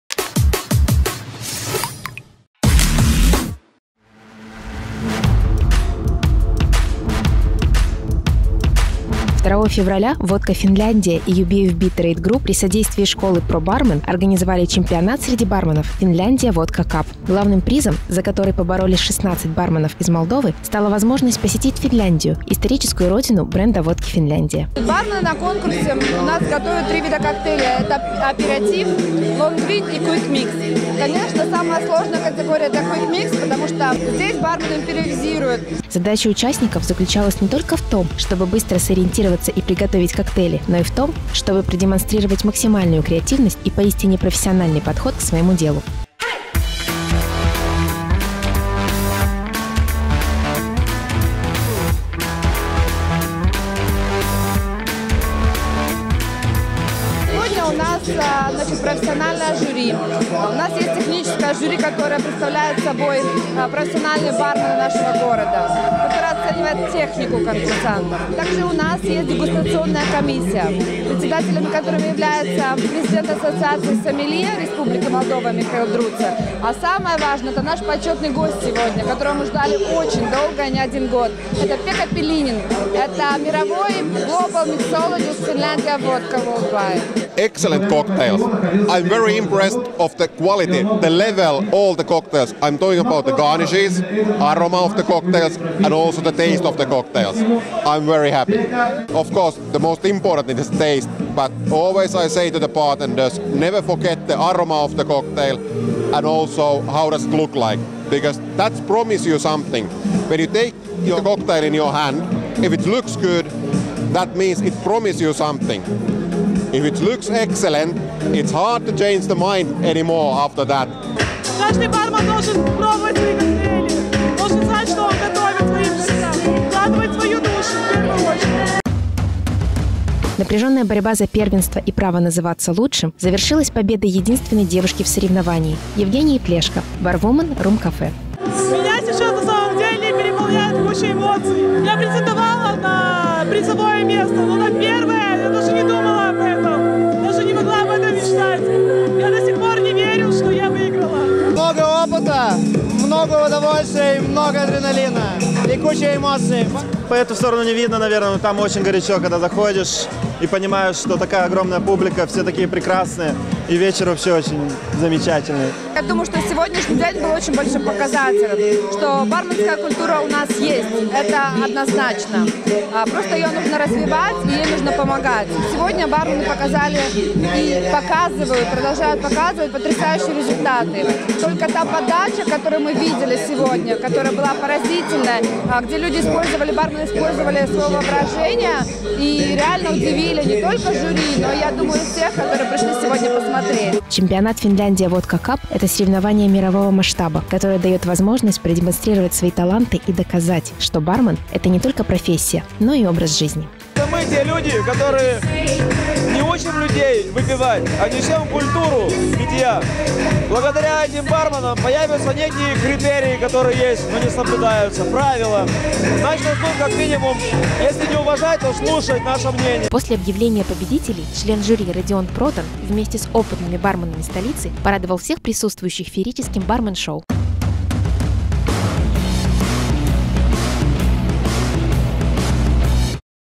Finlandia Vodka Cup 2015
2 февраля  водка Finlandia и UB FB Trade Group при содействии школы PRO Barman организовали чемпионат среди барменов: Finlandia Vodka Cup. Главным призом, за который поборолись 16 барменов из Молдовы, стала возможность посетить Финляндию, историческую родину бренда водки Finlandia.